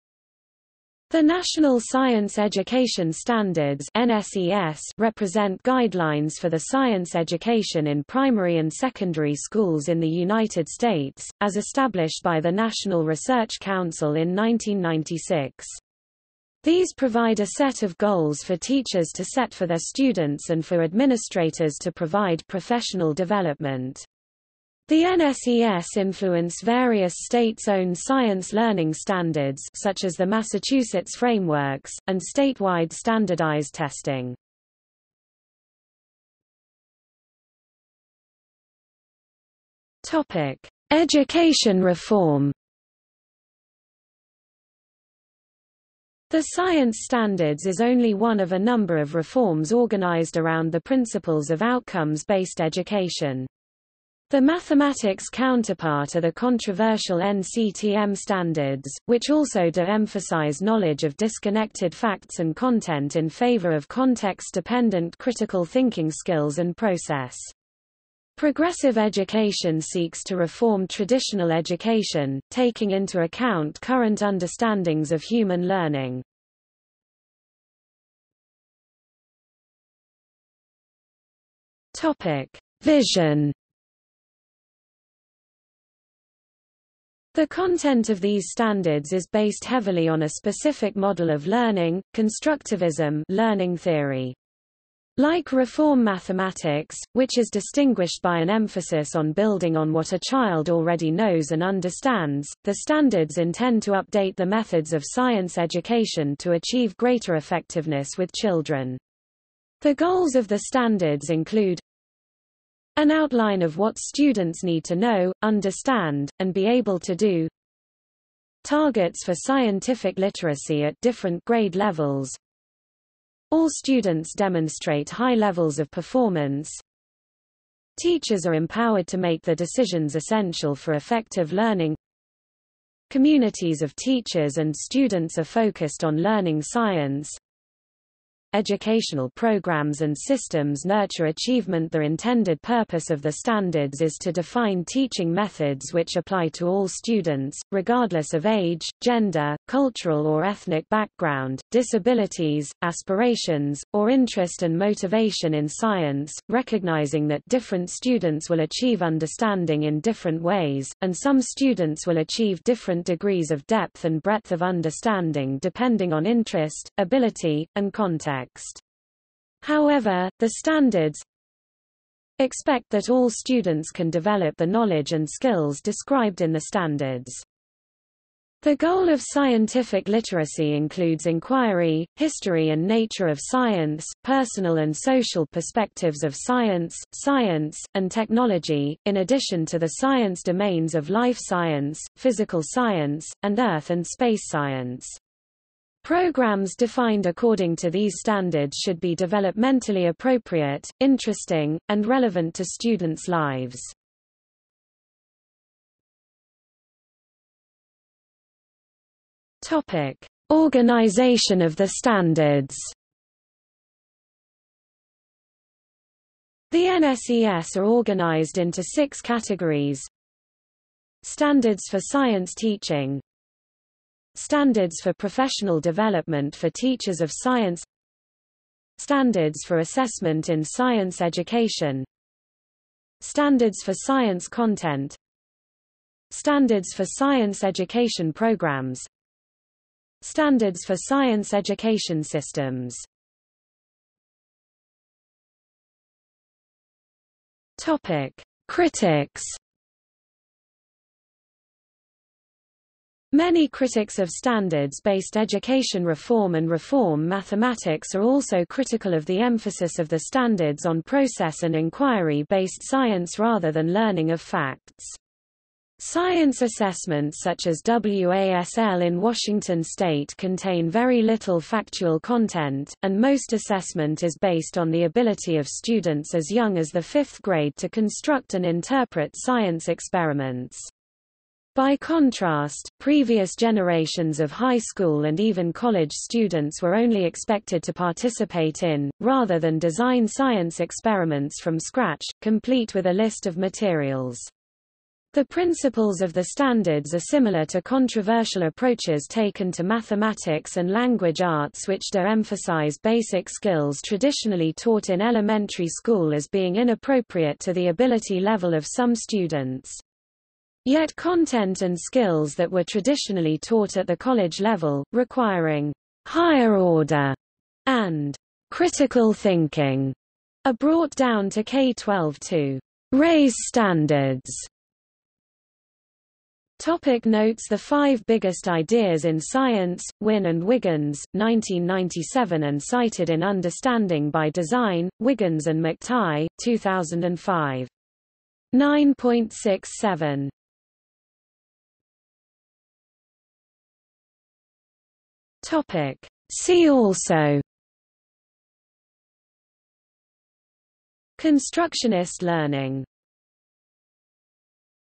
National Science Education Standards | Wikipedia audio article
00:00:36 1 Education Reform
00:01:18 2 Vision
00:03:26 3 Organization of the Standards
00:04:04 4 Critics
00:05:26 5 Notes
00:05:42 6 See also

- Socrates

SUMMARY
The National Science Education Standards (NSES) represent guidelines for the science education in primary and secondary schools in the United States, as established by the National Research Council in 1996.  These provide a set of goals for teachers to set for their students and for administrators to provide professional development.  The NSES influence various states' own science learning standards (such as the Massachusetts Frameworks), and statewide standardized testing.